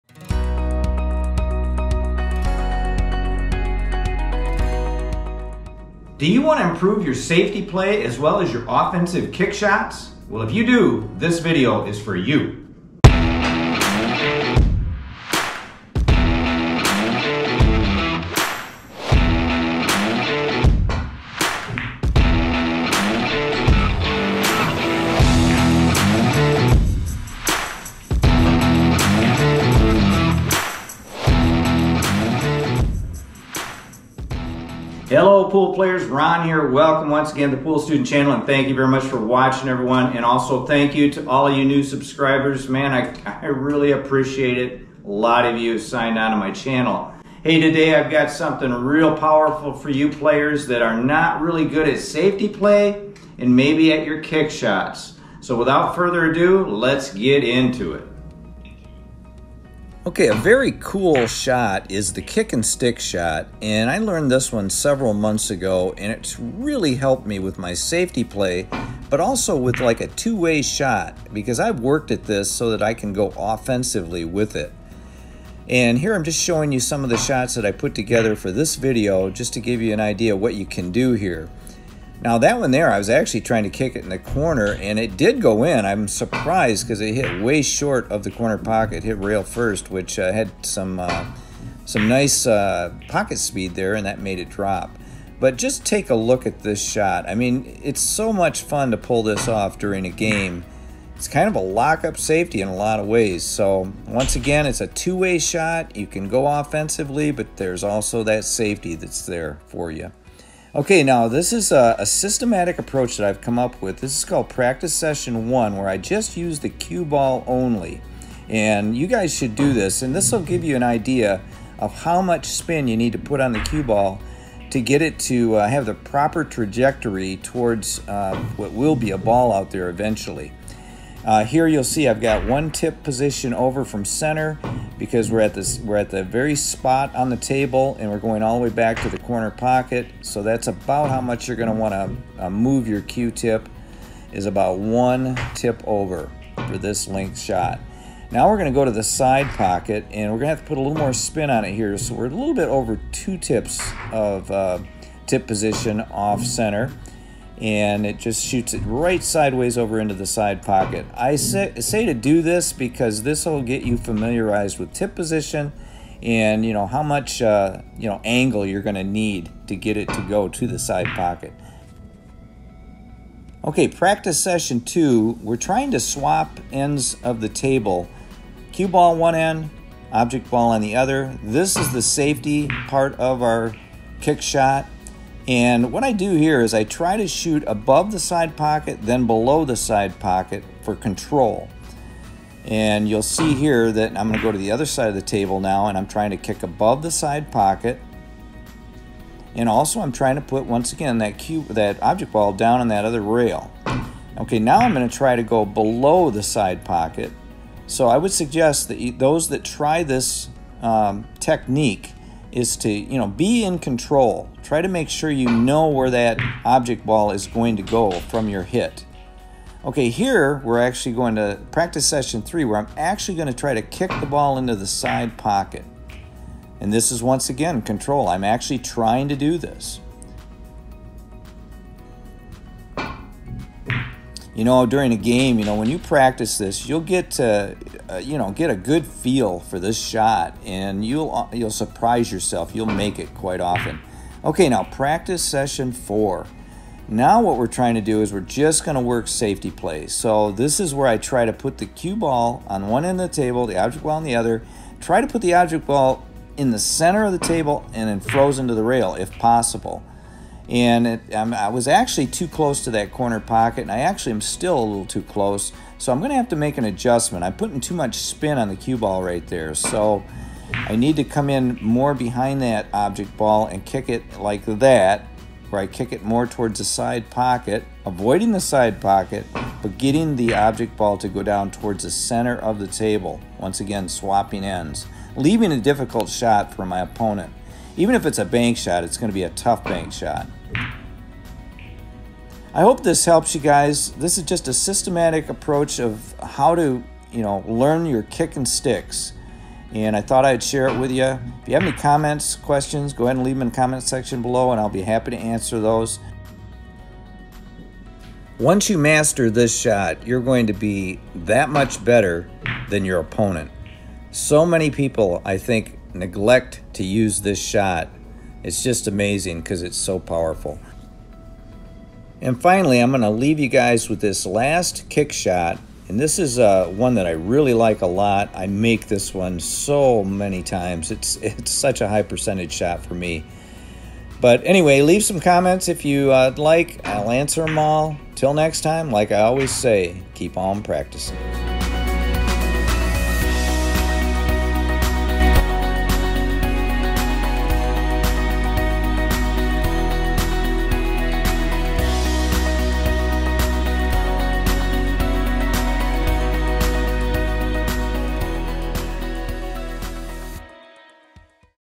Powerful Pool shots Players should know
This video shows a systematic approach in executing a super accurate kick & stick shot for better safety and offensive play.
This simple 4 step system will quickly make this shot one of your go to shots during competition. Learn how to execute strong safeties as well as super accurate kick shots with this system.
Hands down, this one is one of my best exercises to quickly improve your game.